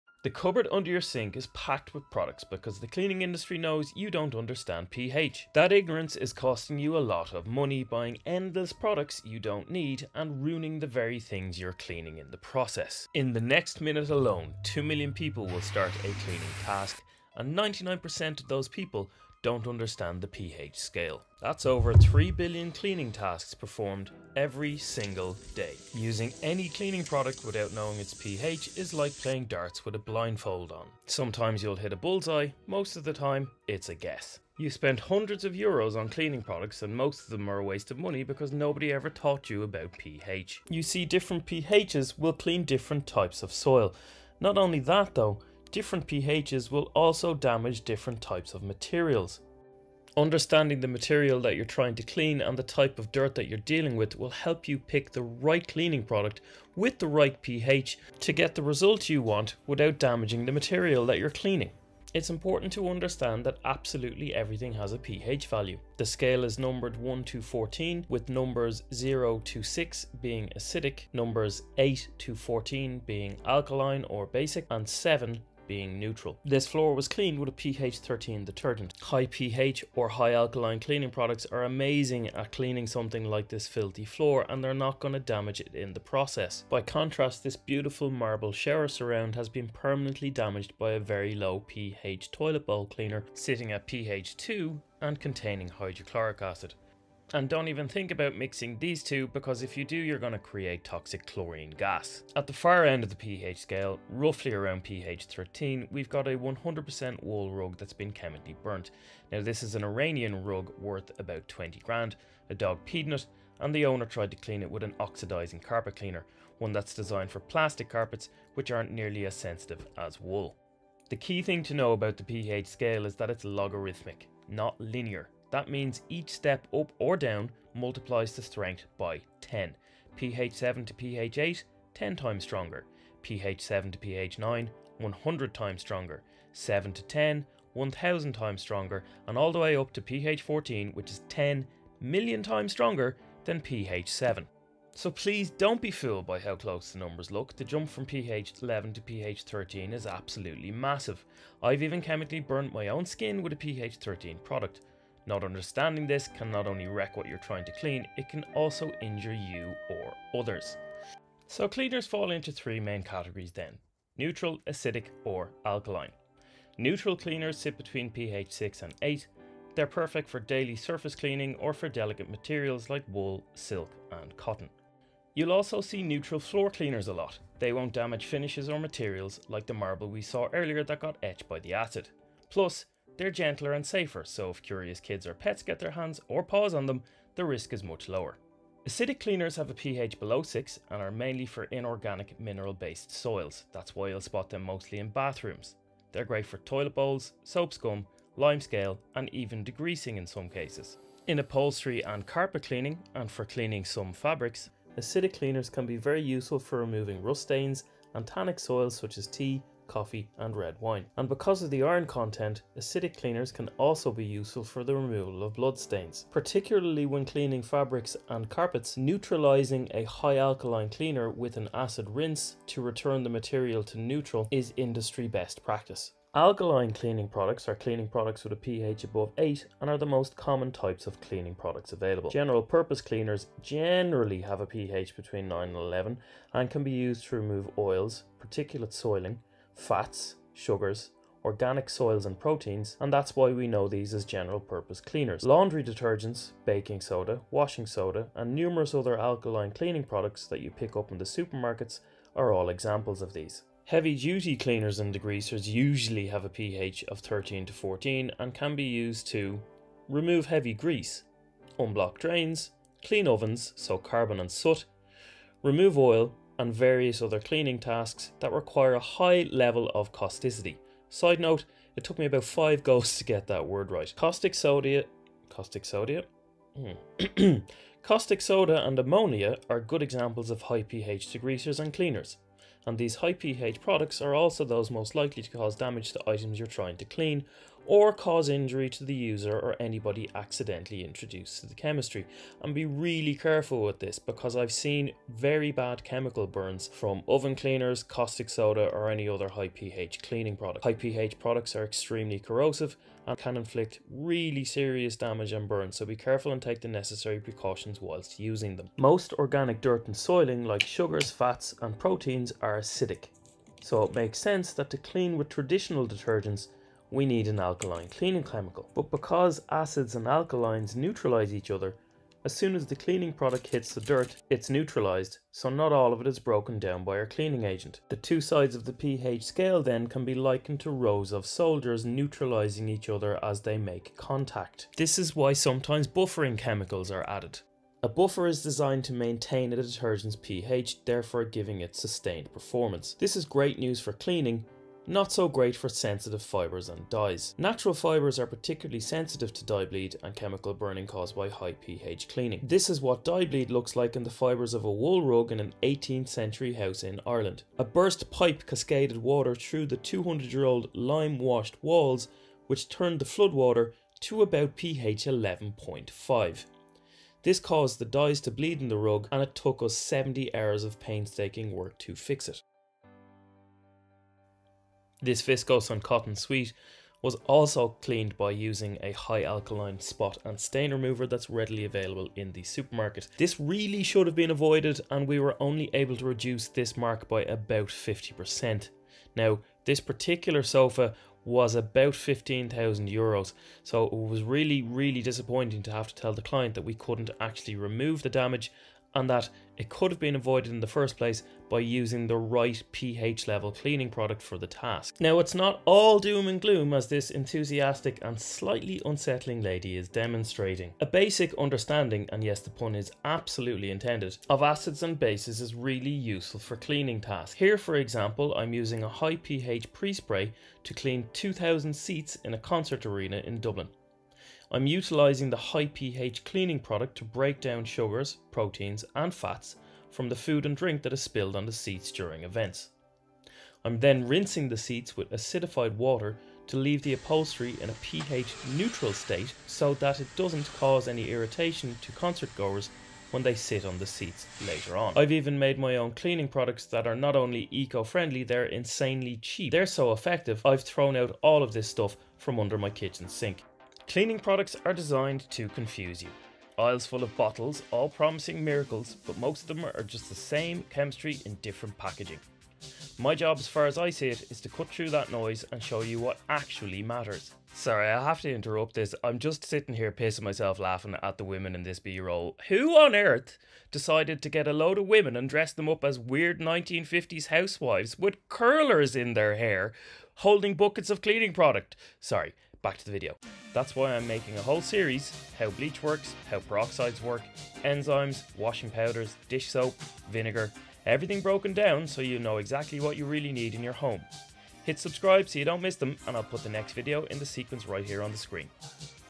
How to Pick the Right Cleaning Product (For ANYTHING).
Before you clean ANYTHING you must understand how PH can affect it so you dont end up damaging your stuff!
Discover how understanding the relationship between **ph and cleaning** can prevent damage and save money. Many common **toxic household cleaners** are misused due to ignorance of **acids and bases**, leading to ineffective cleaning and potential harm. Learn essential **cleaning tips** and the basics of **chemistry** to identify when to use **alkaline** solutions vs acidic ones, and **what are acids and bases**.